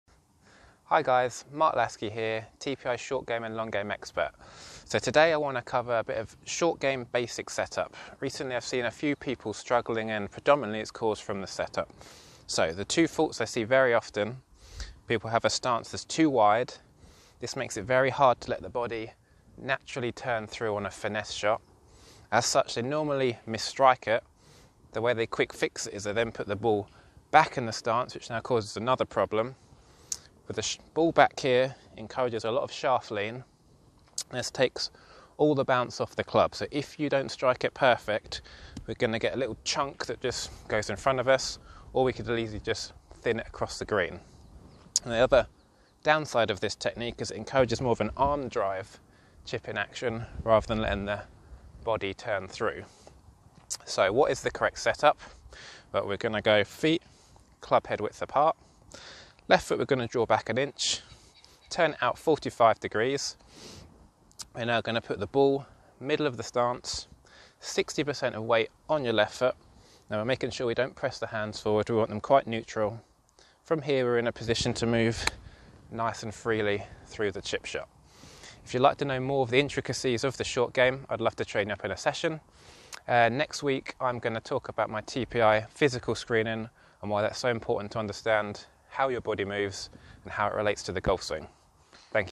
The Academy: Mark Laskey on Chipping
Mark Laskey explains the common mistakes and correct set-up for successful chipping around the green.